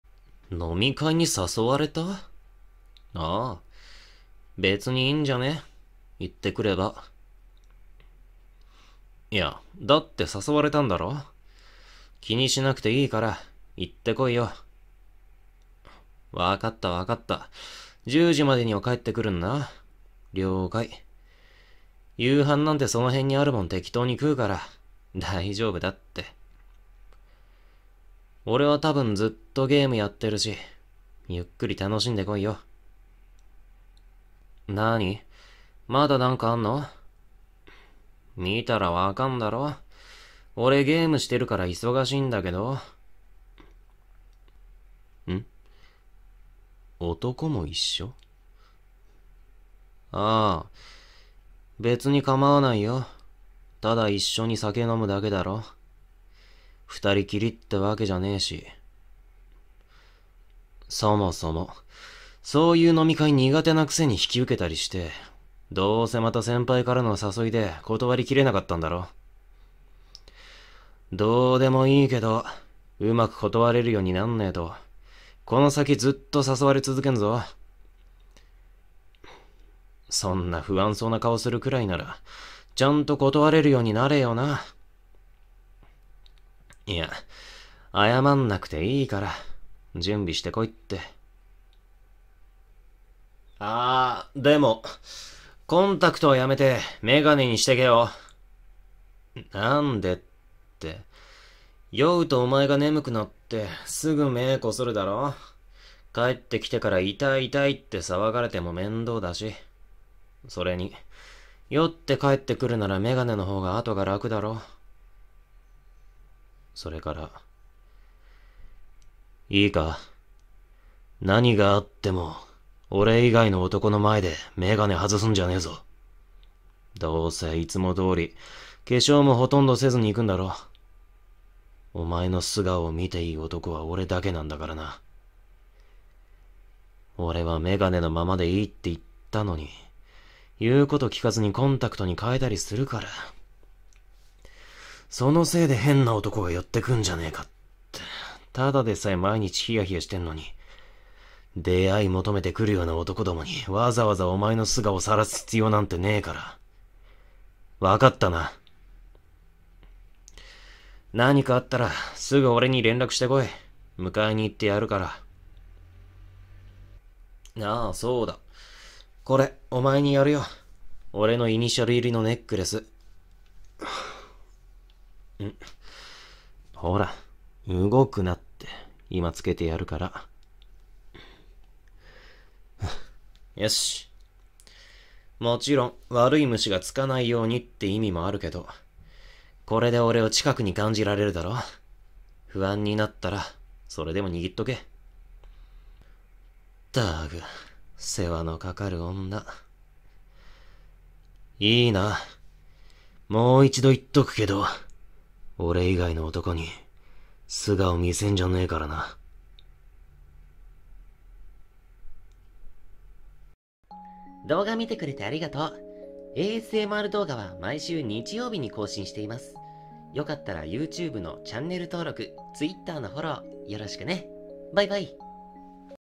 【女性向けボイス】塩系彼氏の隠しきれない独占欲【シチュエーションボイス,バイノーラル】
今回の女性向けボイスは塩対応彼氏が飲み会に行く彼女についつい独占欲を出しちゃうシチュエーションボイスです。

台本：黒背 様
『塩系彼氏の隠しきれない独占欲』

※ASMR動画は毎週日曜日に投稿してるよ